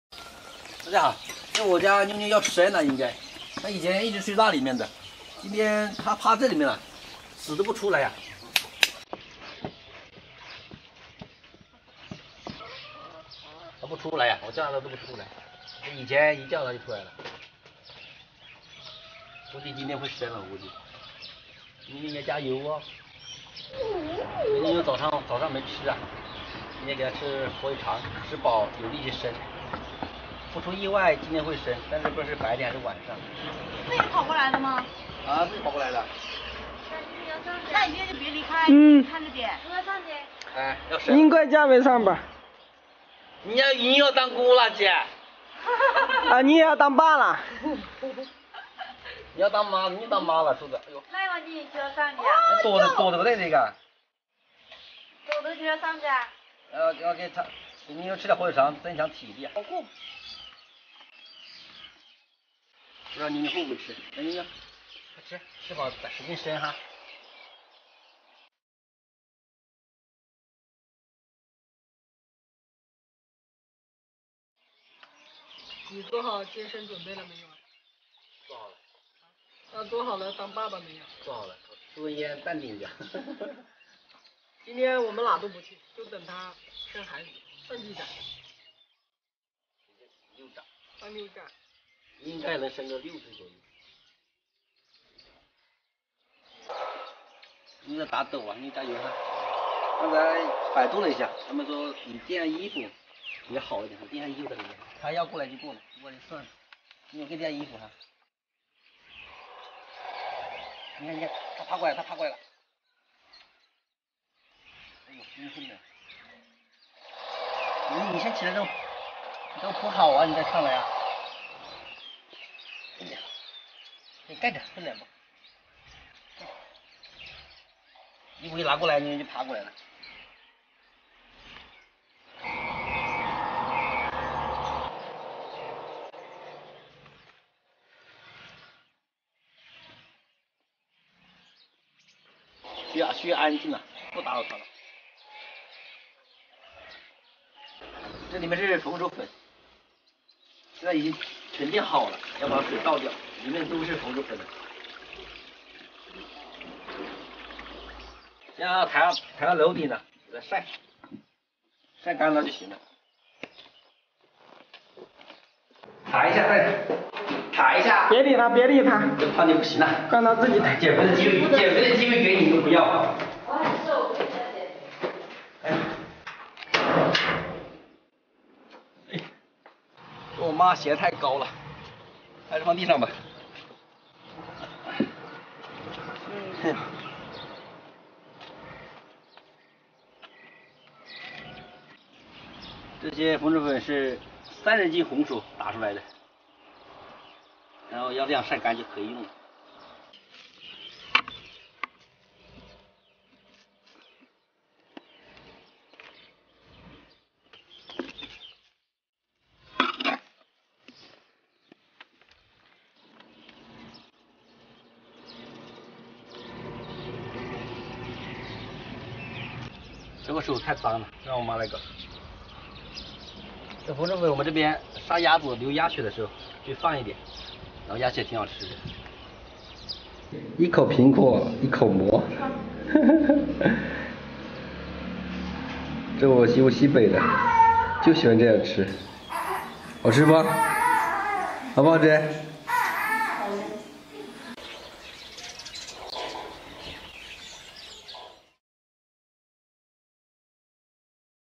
狗狗快生了，農村小哥守著不出門，幫媽媽曬紅薯粉 【石头秀】
我们强调了视频的原创性和安全性，绝对没有任何不良引导或危险行为。 用心创作，感谢大家，开心快乐就好！